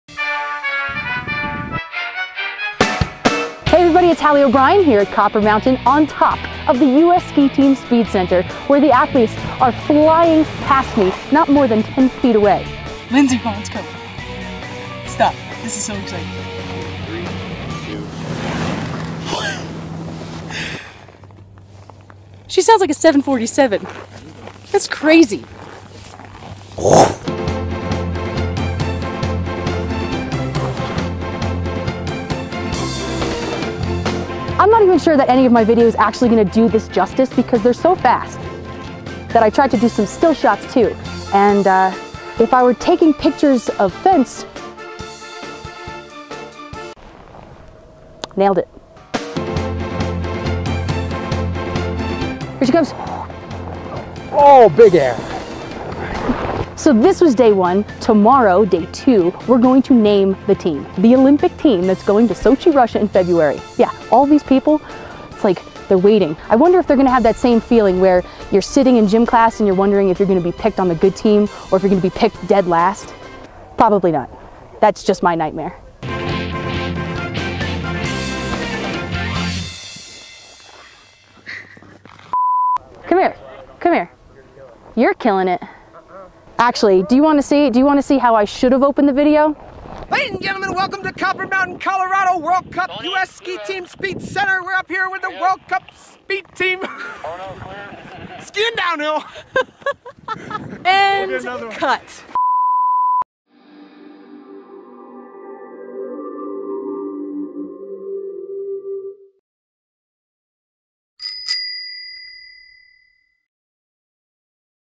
Copper Mountain U.S. Ski Team First Tracks (Day One)
The U.S. Ski Team is fast. In fact, they're so fast, they sound less like skiers and more like a Boeing 747. Halley O'Brien brings us to the U.S. Ski Team Speed Center as the team "jets" by. Bah dum tss.